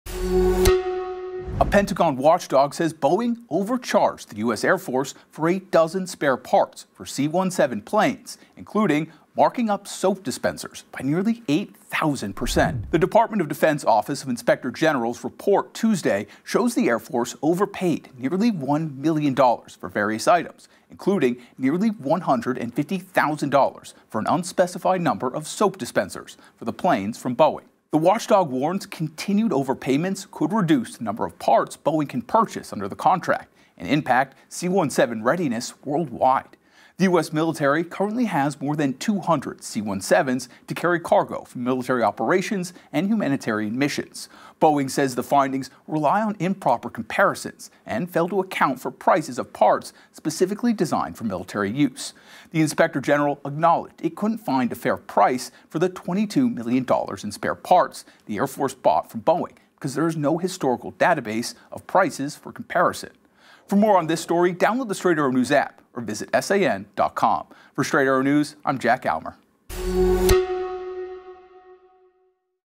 Boeing overcharged US Air Force by nearly $1 million for spare parts: Report
A report from a Pentagon watchdog on Tuesday, Oct. 29, said Boeing overcharged the U.S. Air Force for a dozen spare parts for C-17 transport planes. That included marking up soap dispensers by nearly 8,000% for the aircraft.

The U.S. Department of Defense Office of the Inspector General’s report shows that the U.S. Air Force overpaid nearly $1 million for various items, including nearly $150,000 for an unspecified number of soap dispensers for the planes from Boeing.

The watchdog warned that continued overpayments for spare parts could reduce the number of parts Boeing is able to purchase under the contract that runs through 2031 and impact C-17 readiness worldwide.

The U.S. military currently has more than 200 C-17s to carry cargo for military operations and humanitarian missions.

Boeing responded by saying the findings in the report rely on improper comparisons and fail to account for prices of parts specifically designed for military use.

The inspector general acknowledged that it could not find a fair price for the $22 million in spare parts the Air Force bought from Boeing because there is no historical database for prices for comparison.